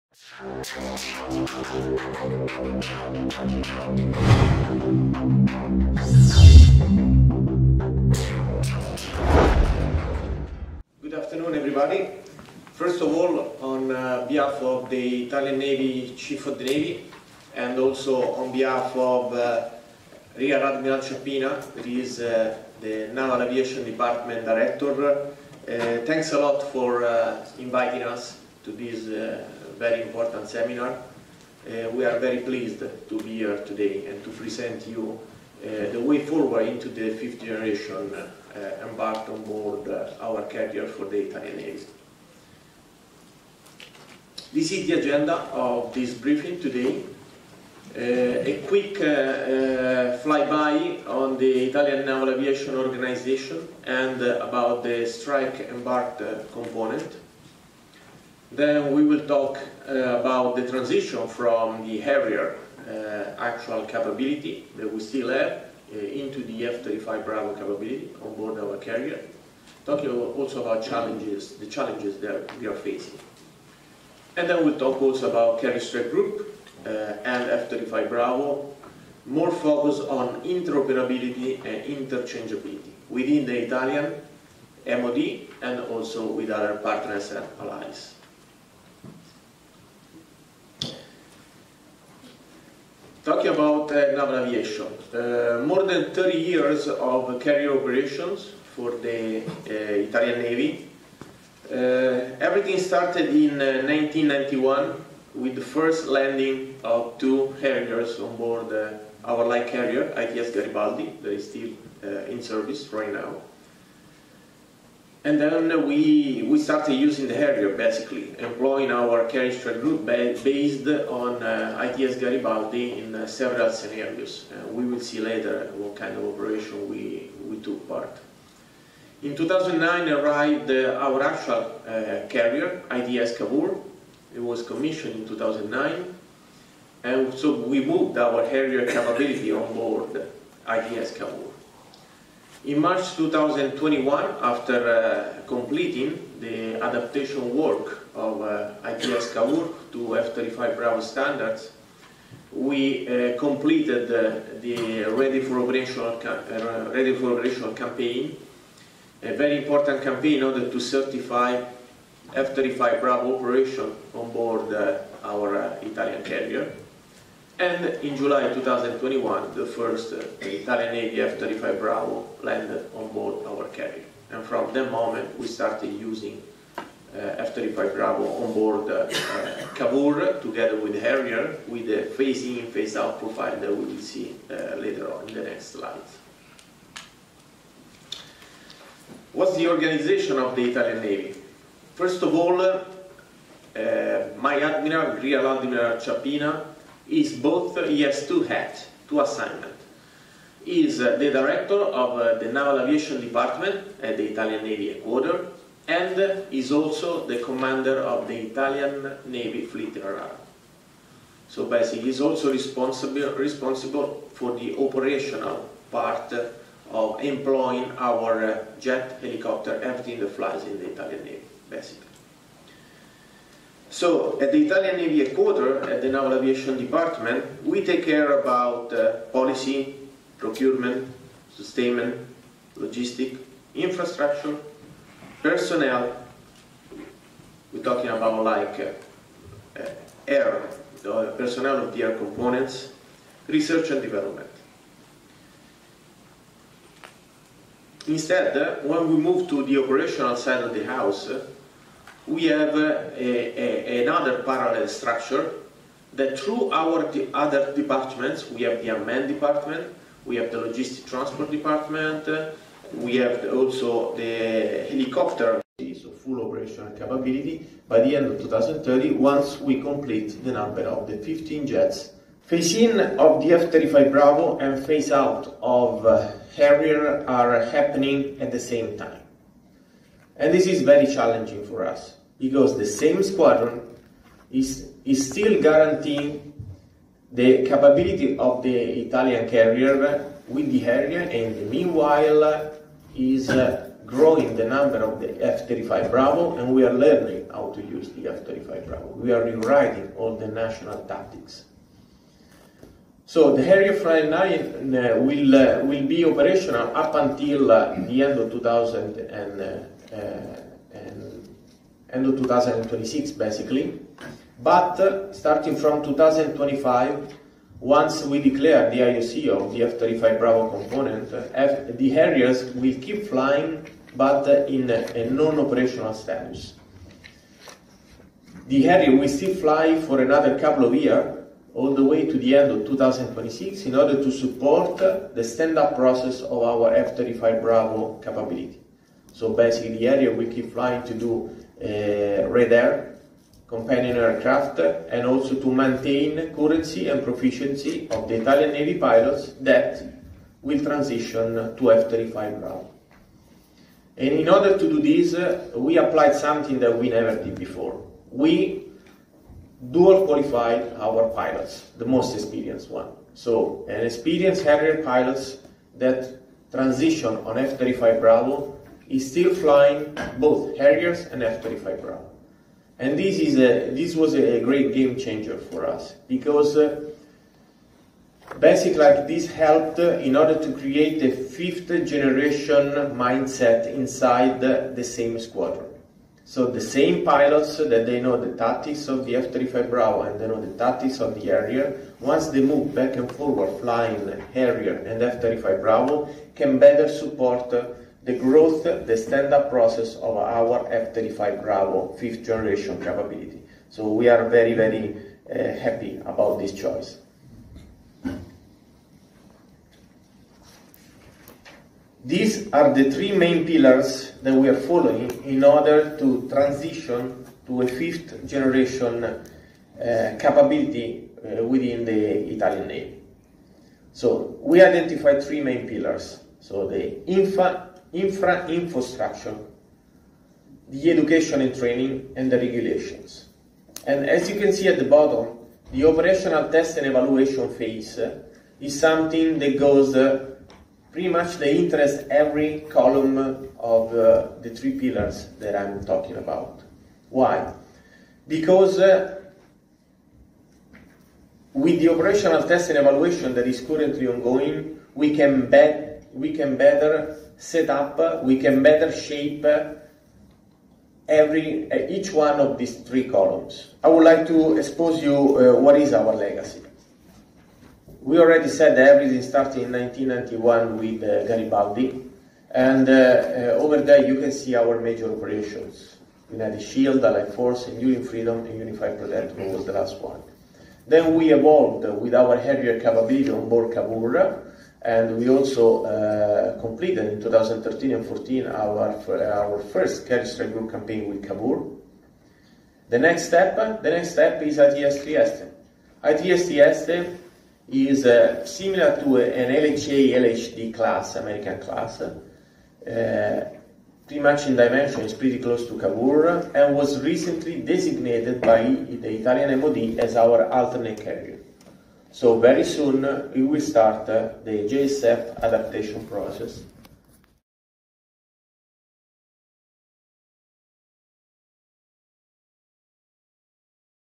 “이탈리아 해군의 스텔스 5세대 함재기 운용에 대하여”
이탈리아 해군본부 항공국의 Fabio Marzano 대령 발표